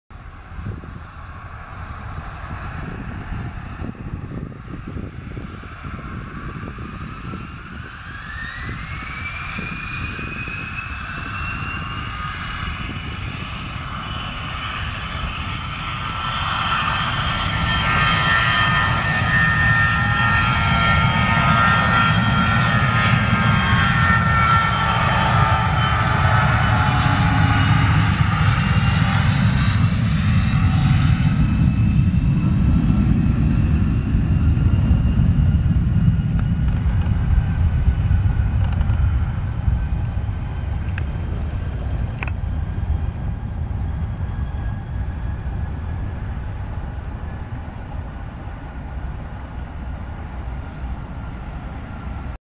Georgian CRJ100 take off, Dtw*
Air Georgian operating for Air Canada Express Bombardier CRJ100, C-FSKM, departing from Detroit metro, dtw runway 21R